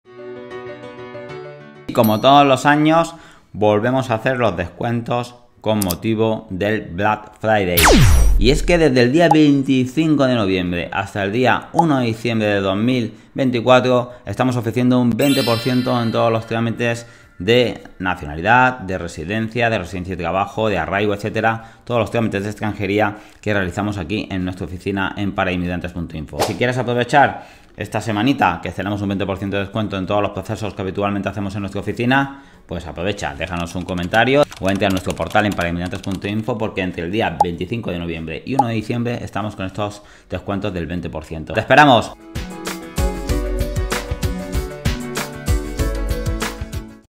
⚫️ 🛒 DESCUENTOS en Extranjería y Nacionalidad 🛒 ⚫️ ¡Black Friday en Parainmigrantes.info!
⚫️ ¡Llega el BLACK FRIDAY a Parainmigrantes.info con ofertas exclusivas para ti! ✨ Ahorra un 20% contratando ahora cualquier trámite de extranjería o nacionalidad hasta el 1 de diciembre ✅ ¡Es tu momento! 🛒 ¡Corre, que una semana pasa volando! 😉 👉🏼 Rellena el formulario que verás en este enlace y te contactamos sin compromiso ➡️

🙋🏻‍♂️ Recuerda que el nuevo reglamento de extranjería no entrará en vigor hasta el 20 de MAYO de 2025, por lo que a partir de esa fecha podremos iniciar solicitudes de los nuevos permisos. ¡Hazlo al mejor precio contratando tu trámite durante esta semana! 👍🏼